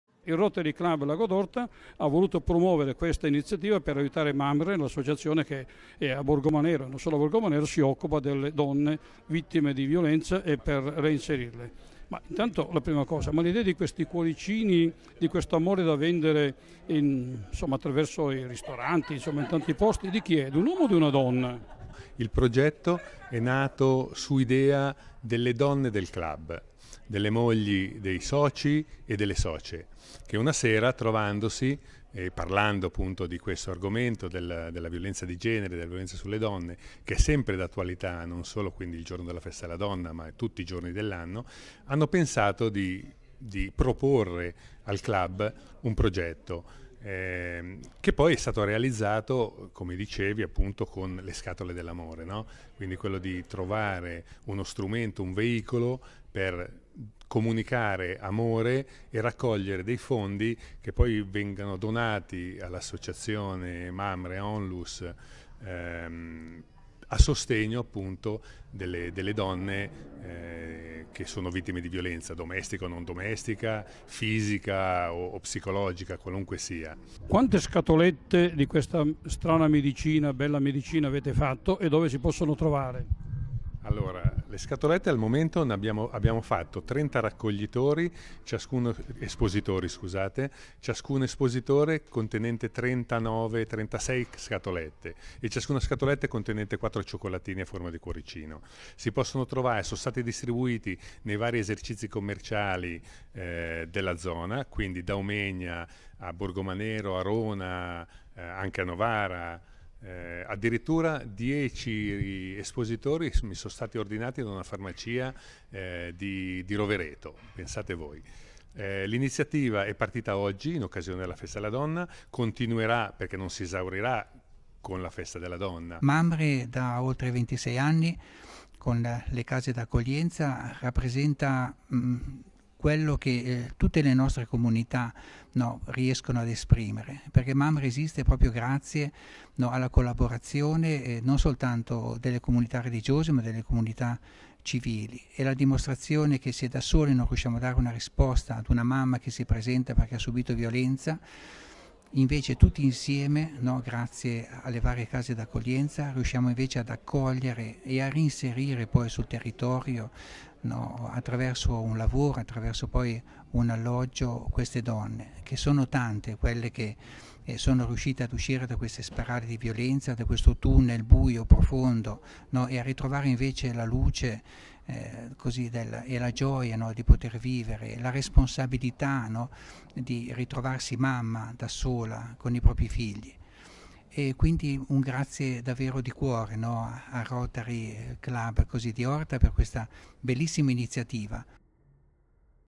"Amore" è il progetto promosso dai club Rotary del Cusio e borgomanerese
9 marzo 2022 - Il progetto si chiama “Amore” ed è promosso dal Rotary Club Orta San Giulio in collaborazione con il Rotary Club di Borgomanero Arona. Scopo : ridare loro fiducia e sicurezza a chi è vittima di violenza. La proposta è semplice: acquistare le scatole  AMORE partendo da una donazione di almeno 3 euro, il cui ricavato sosterrà le attività di MAMRE. Medicine speciali ovvero  4 cioccolatini ripieni di solidarietà ed un bugiardino nel quale vengono raccontate delle storie reali di donne che sono riuscite ad uscire dalla spirale dell’amore malato.